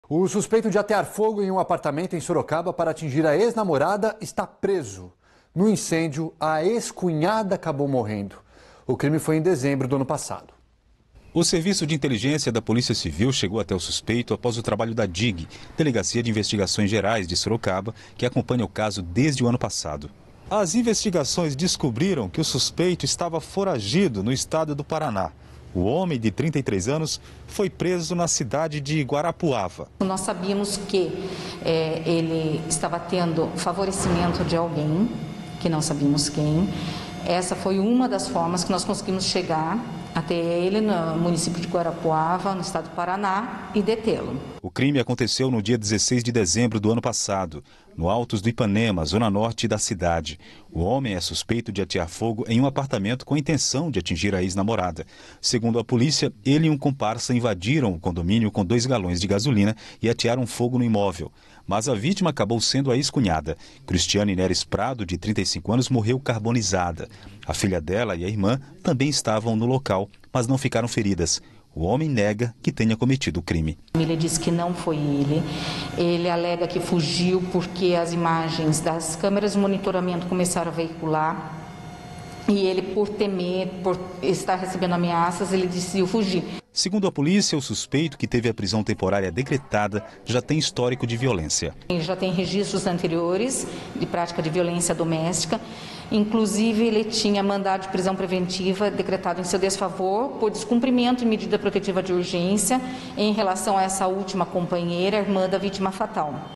Preso suspeito de atear fogo em apartamento de Sorocaba | TV Sorocaba SBT
Preso suspeito de atear fogo em apartamento de Sorocaba.

O suspeito de atear fogo em um apartamento em Sorocaba pra atingir a ex namorada está preso. A ex-cunhada do homem morreu no incêndio.

Sorocaba
Fogo
Apartamento